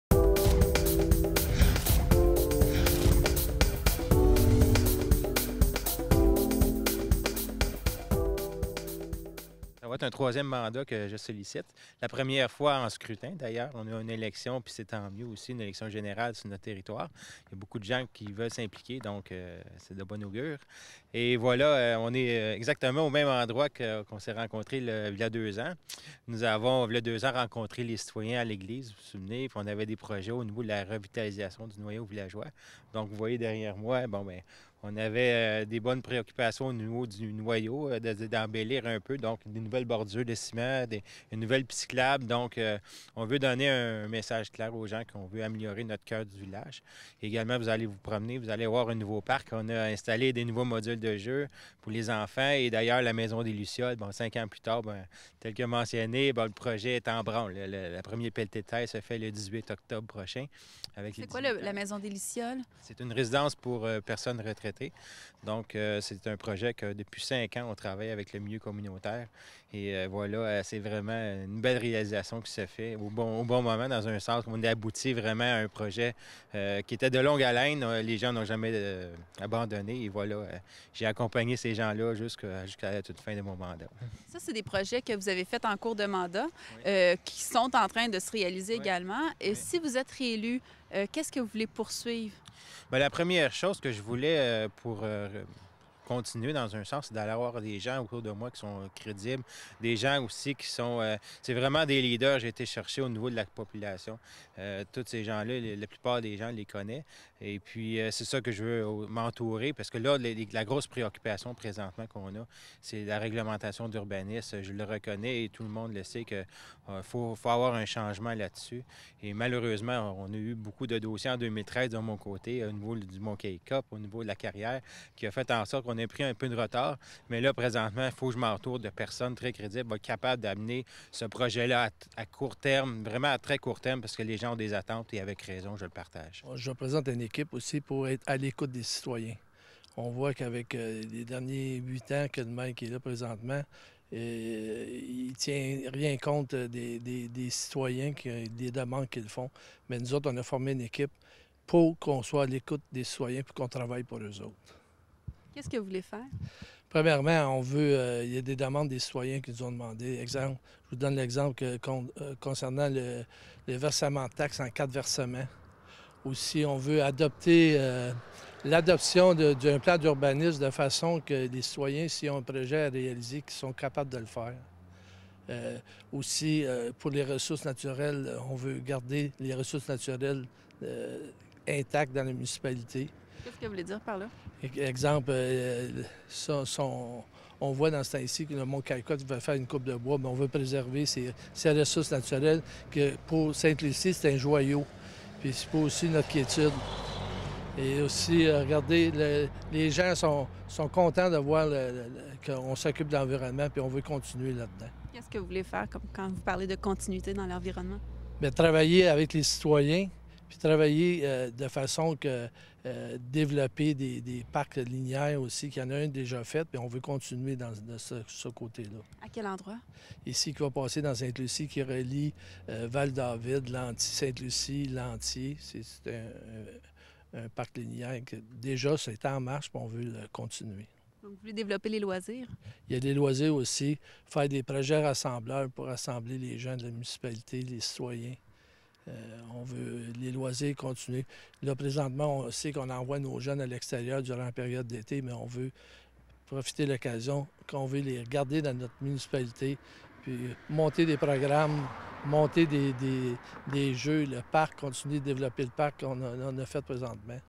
Ste-Lucie
Présentation des candidats à la mairie de Ste-Lucie-des-Laurentides pour les élections municipales du 3 novembre 2013.